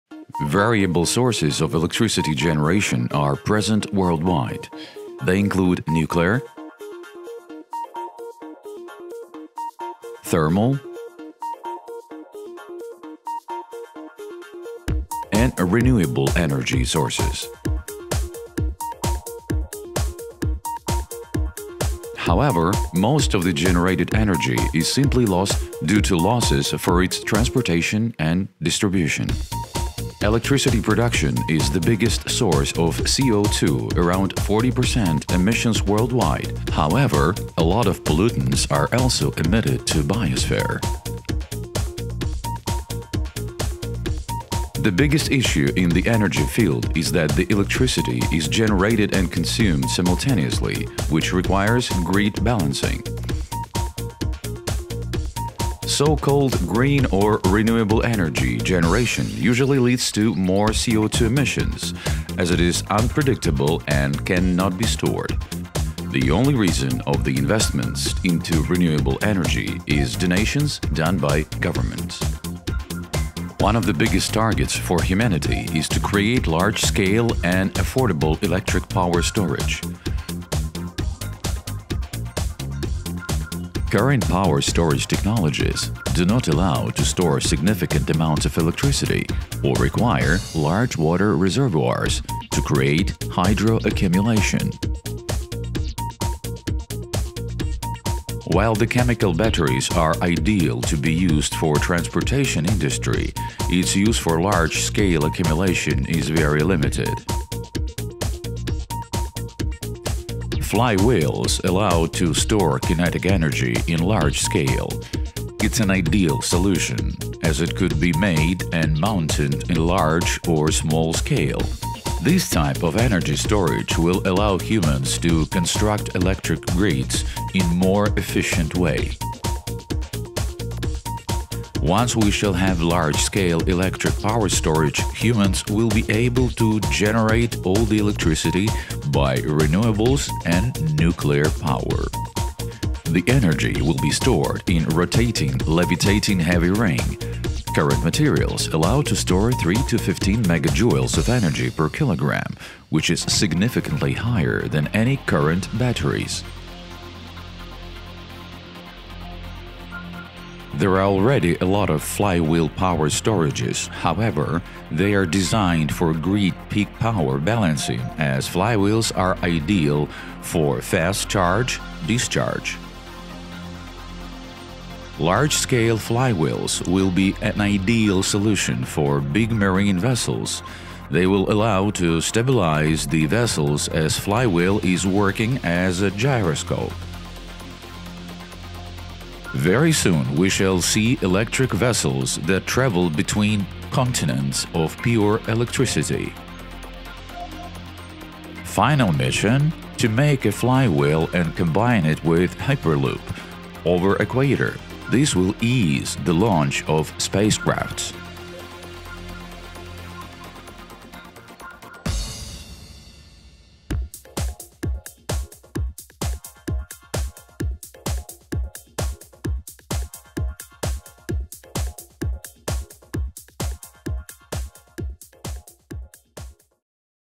How flywheel energy storage system works
Our flywheel energy storage system with magnetic levitation technology will be a game-changer in the energy storage market. Together with partners COR-Energy is developing E-NER-COR,  flywheel energy storage system that can reduce frictional losses and increase efficiency, resulting in longer  rotation time and be effective as an energy storage.